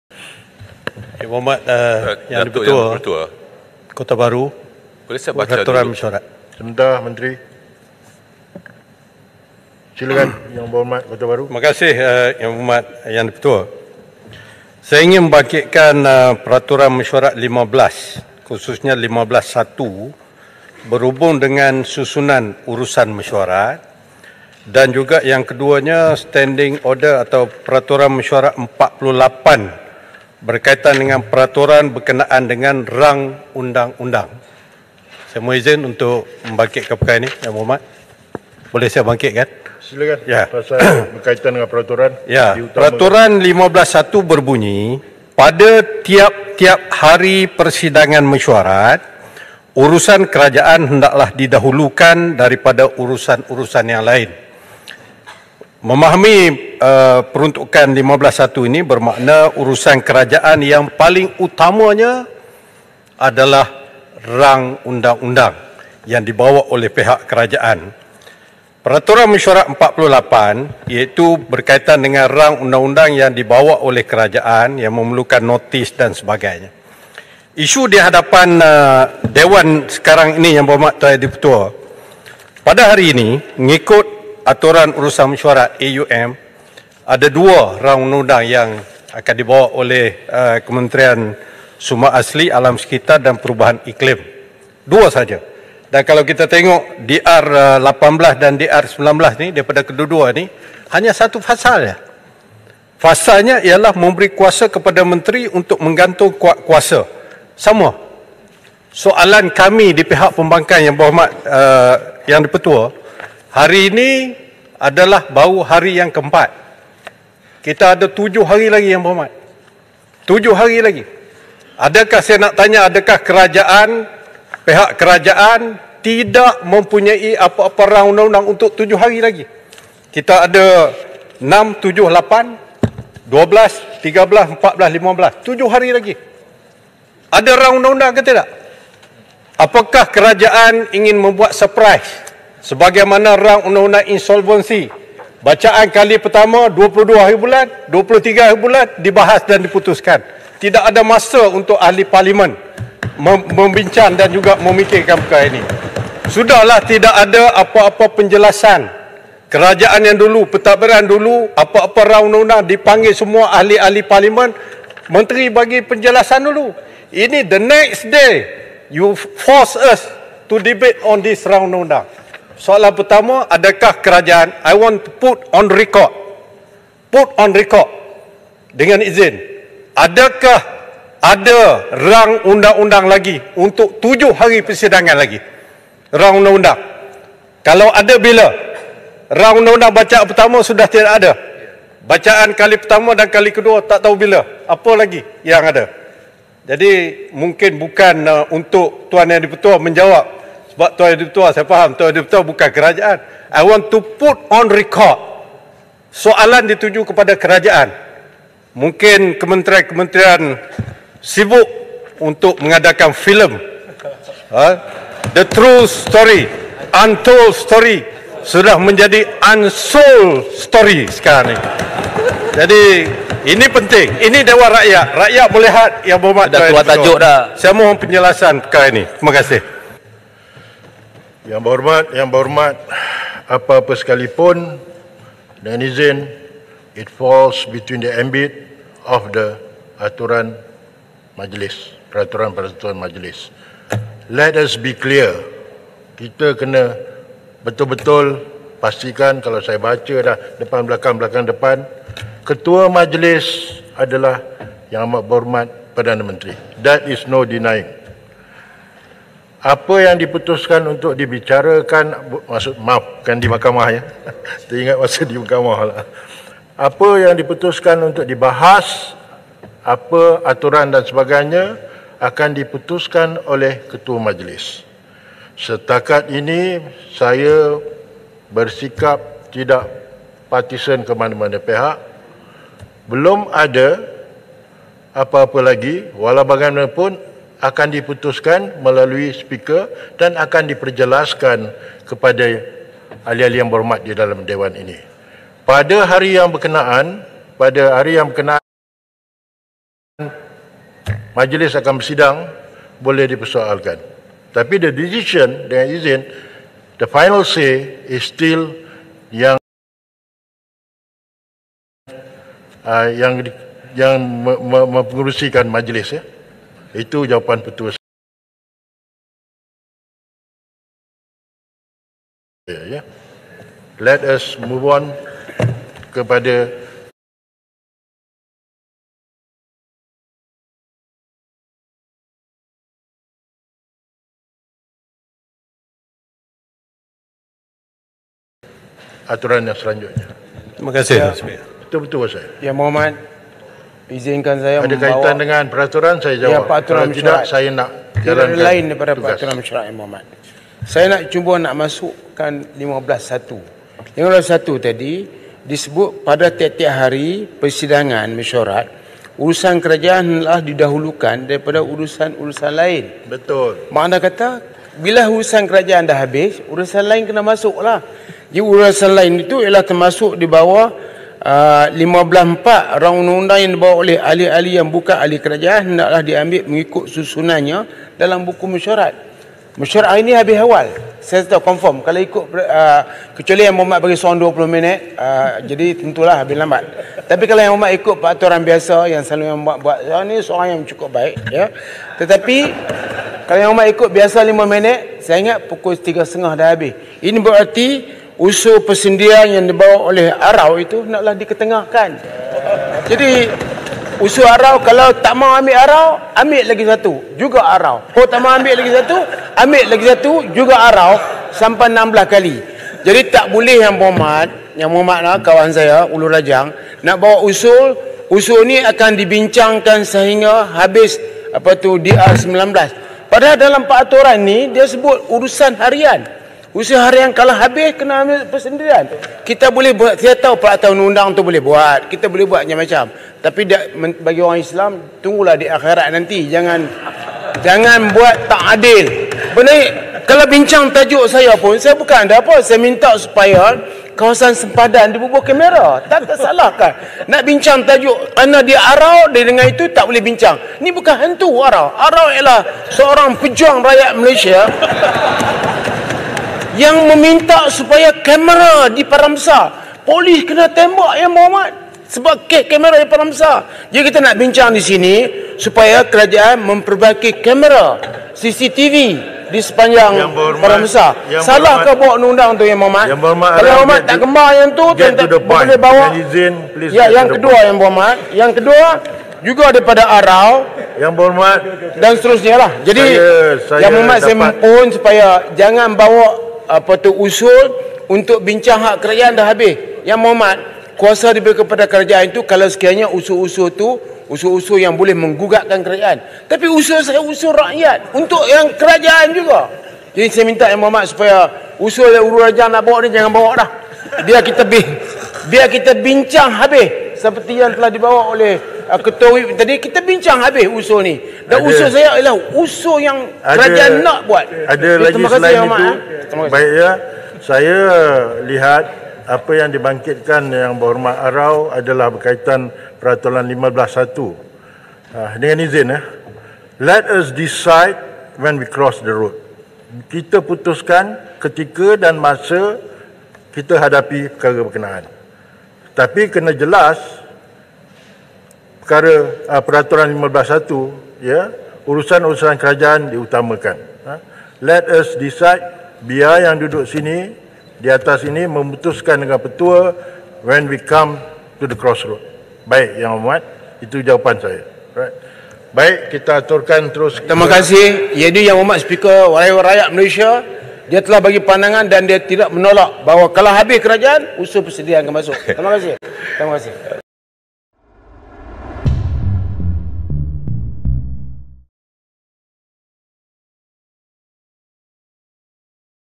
Opposition criticises government for absence of new Acts and amendments
The opposition today needled the government over an apparent lack of bills to amend laws or legislate new Acts in the Dewan Rakyat's order paper.

This is after the government only had two very short bills on the order paper today and nothing else.

The bills to amend the Renewable Energy Act and Sustainable Energy Development Authority Act both contain just one amendment, which is to allow the minister in charge to suspend the operation of the whole Act(s) or parts of it in any part of the country.

Takiyuddin Hassan (Perikatan Nasional-Kota Bharu) questioned why the government had nothing else lined up when the current Dewan Rakyat meeting still had seven more days to go.